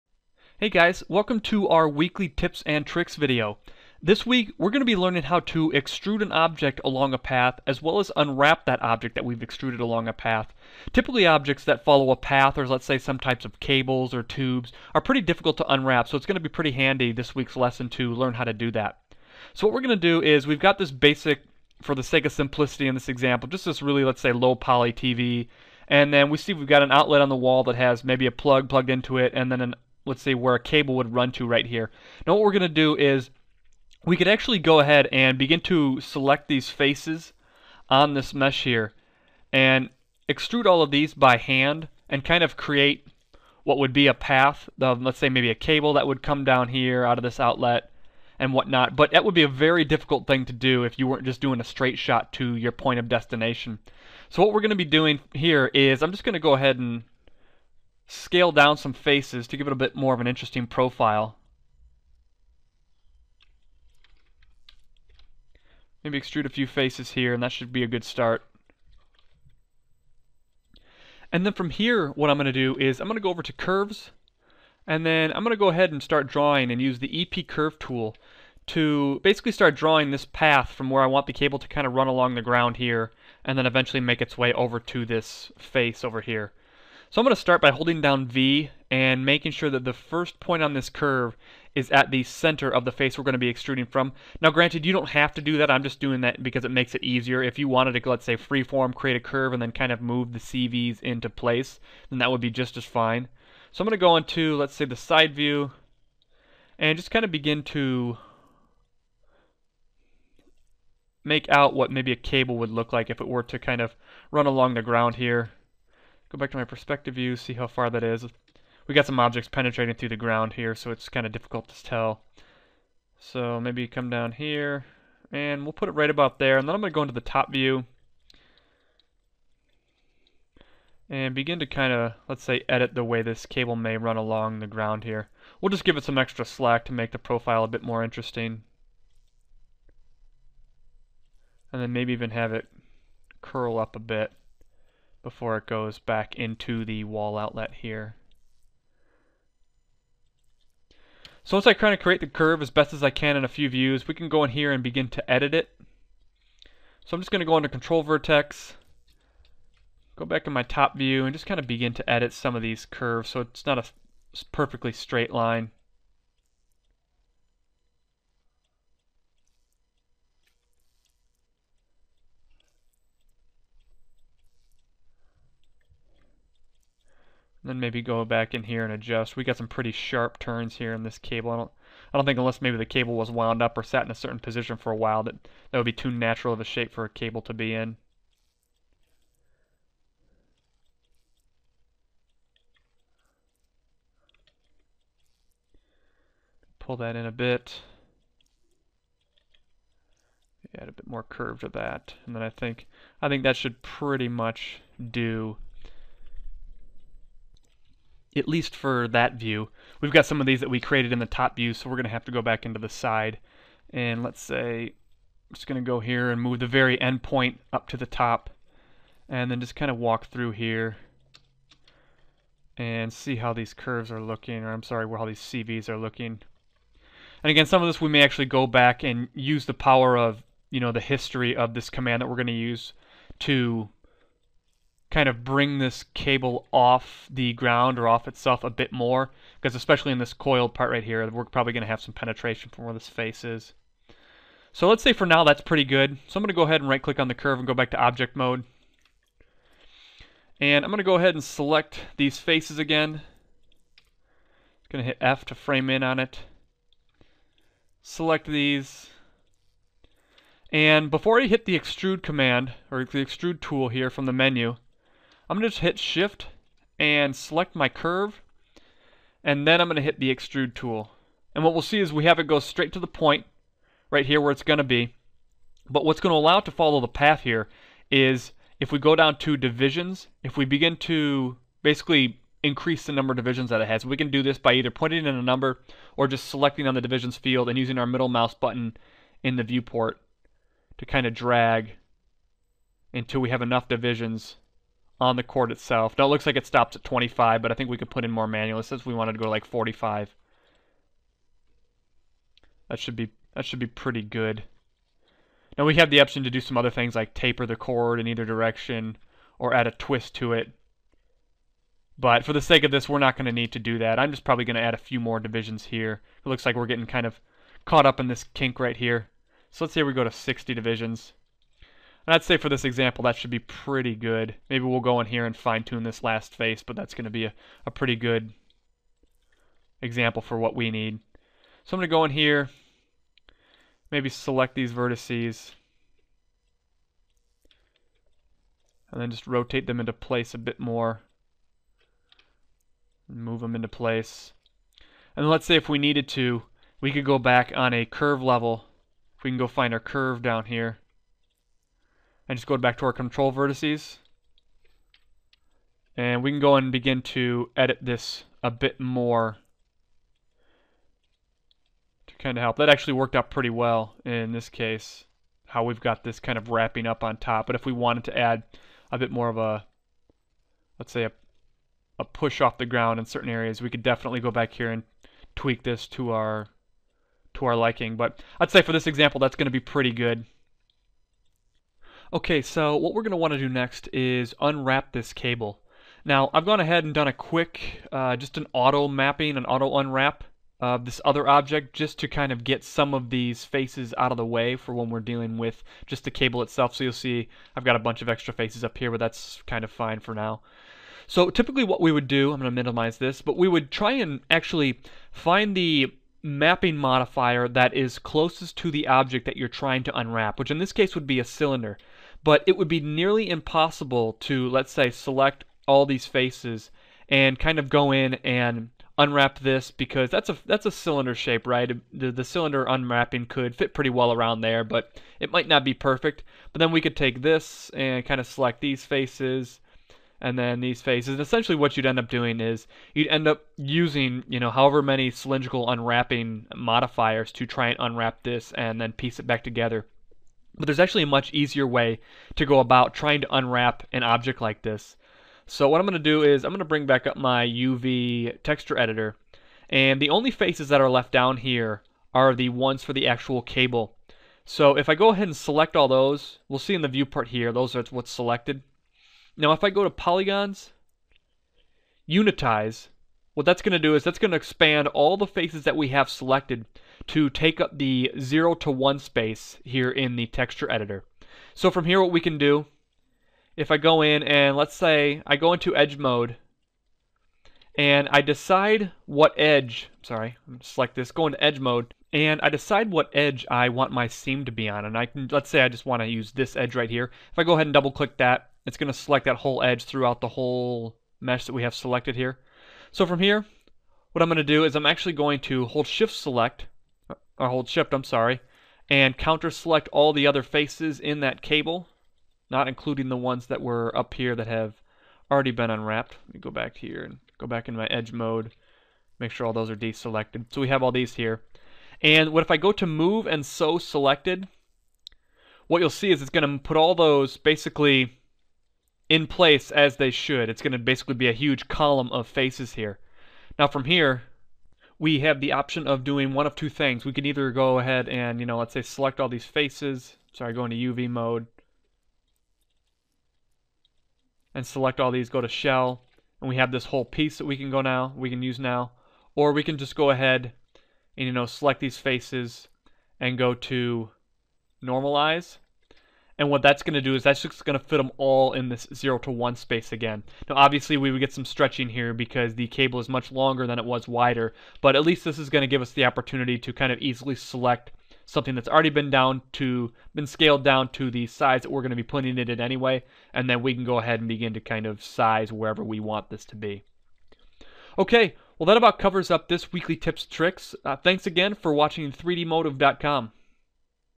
Path Extrusion and Unwrapping - 3dmotive
In this lesson, we will be learning how to extrude geometry along a path to create a cable as well as learn how to unwrap any object that has been created using a similar method.